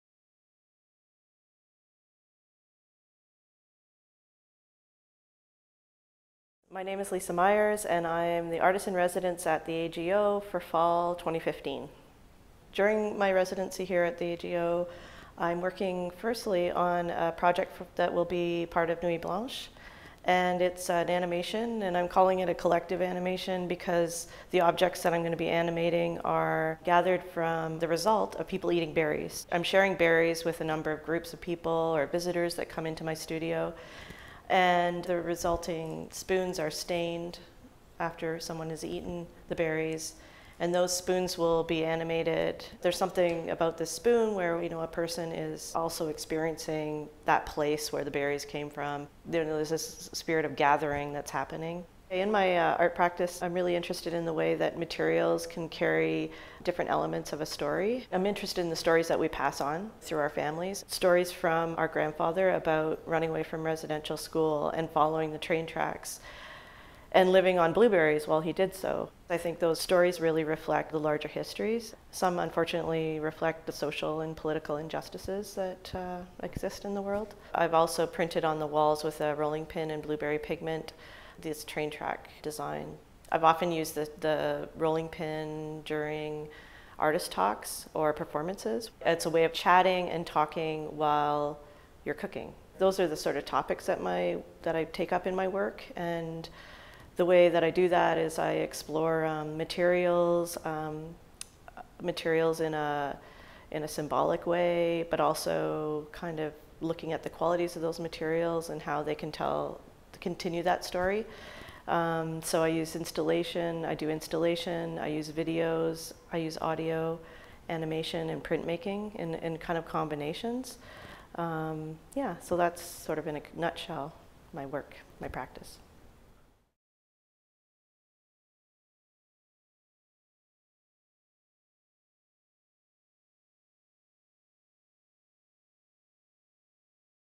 Artist-in-Residence Lisa Myers
Fall 2015 Artist-in-Residence, Lisa Myers discusses her installation for Nuit Blanche.